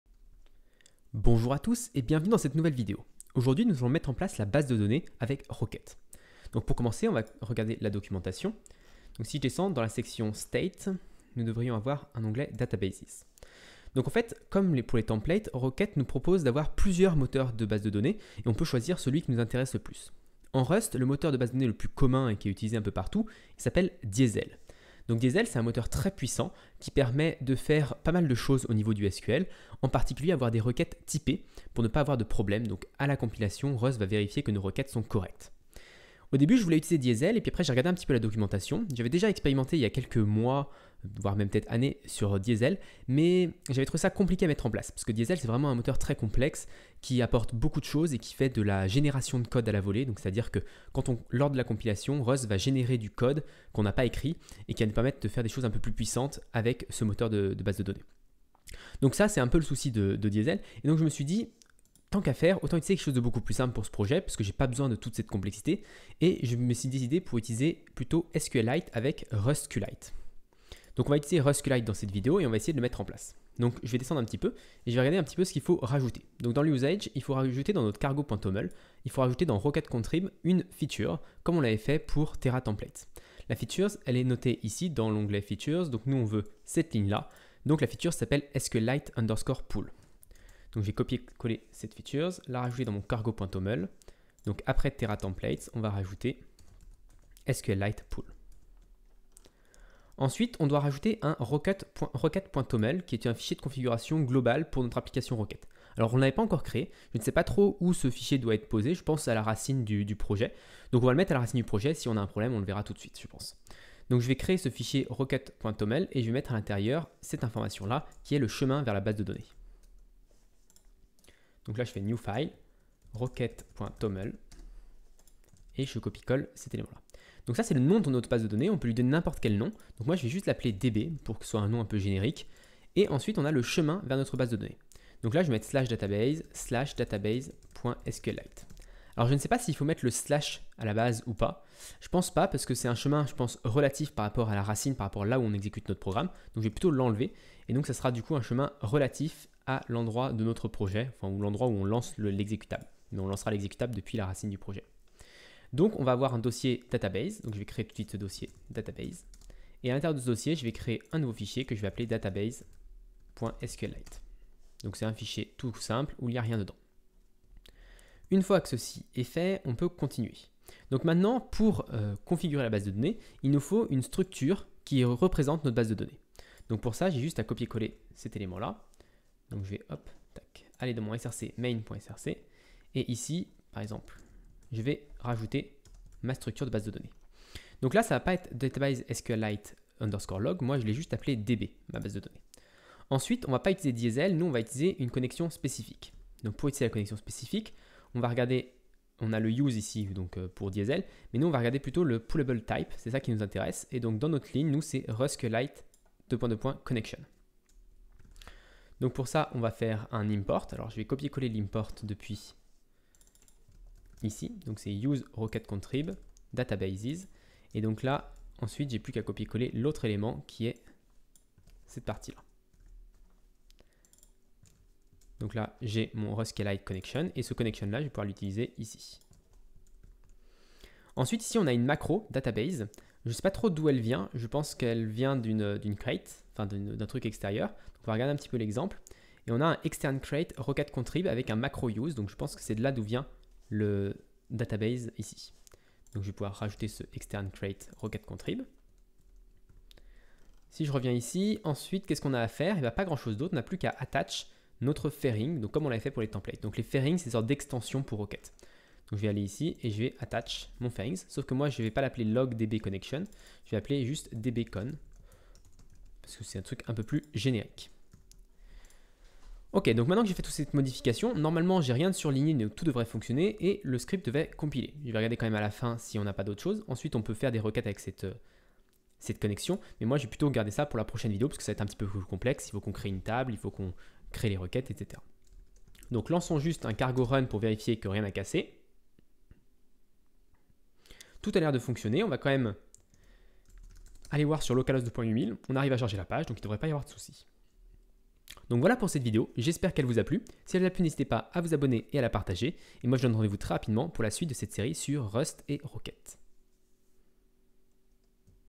05 - Configuration de la base de données SQLite - Développement Web Rust & Rocket
Dans cette vidéo je configure Rocket pour utiliser une base de données SQLite.

si vous avez des questions sur le développement web.